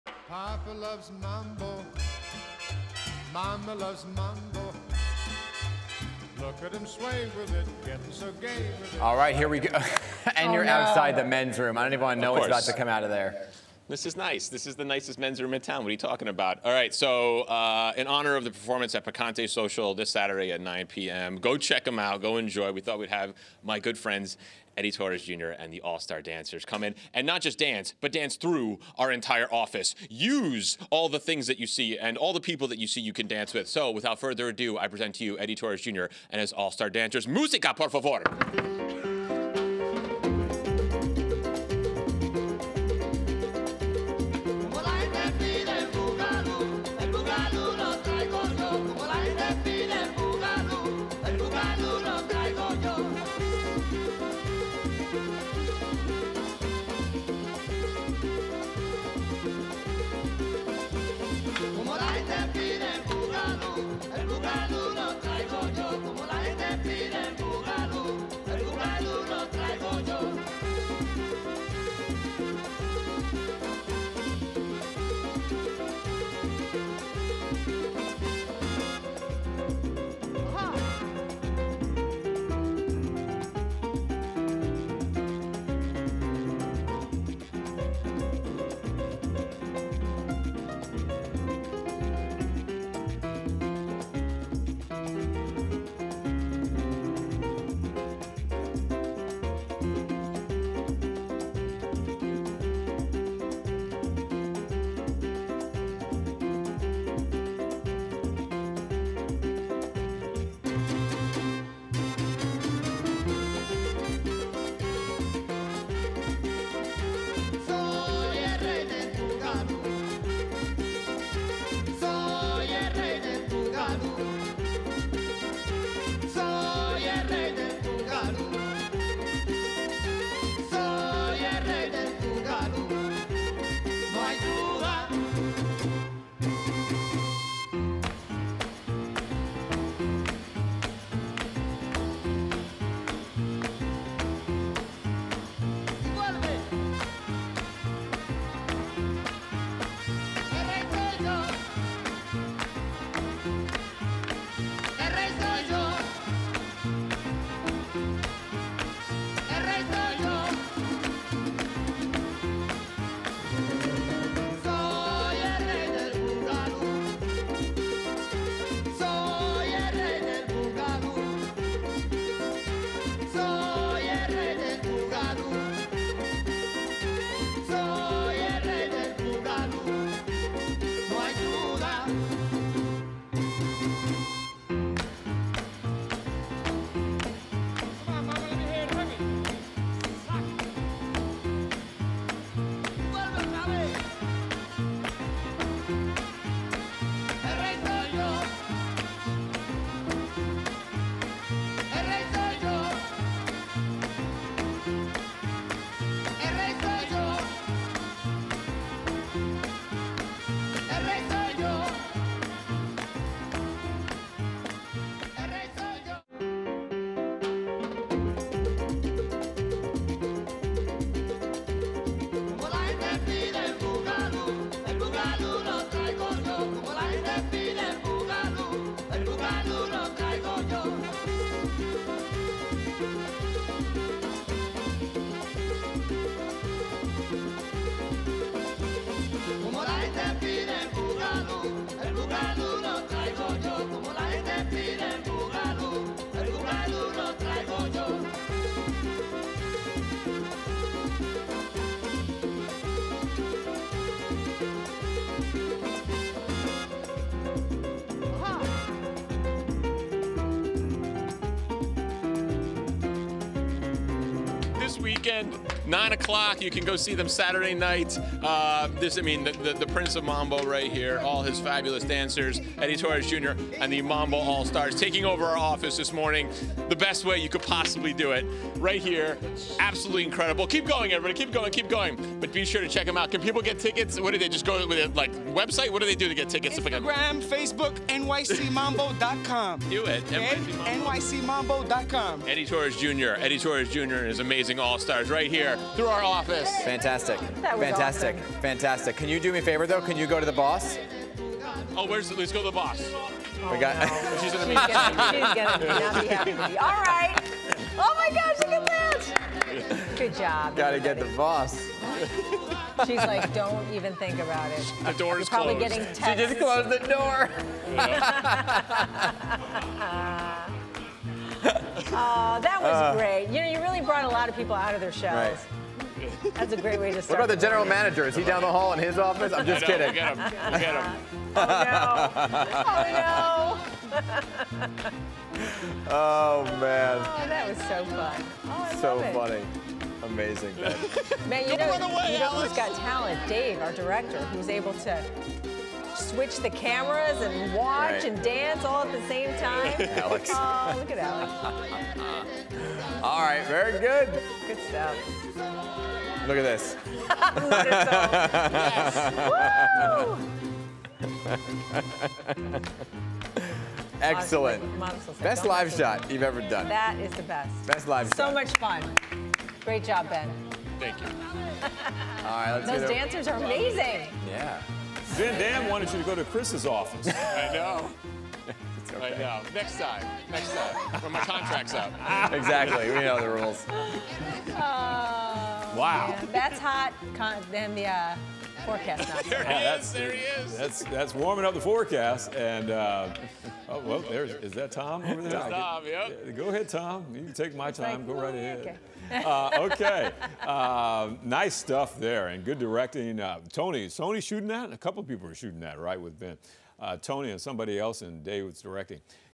Dance the mambo in the PIX11 Newsroom
Ben Aaron shakes up the PIX11 Newsroom with the help of mambo dancers.